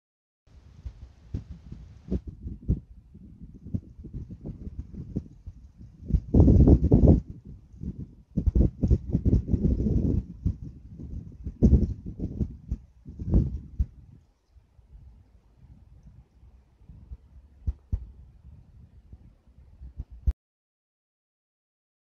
Saturday's snow at the house
NWF snowfall from the upper low on Saturday in E. Asheville. Around 5 inches on the ground.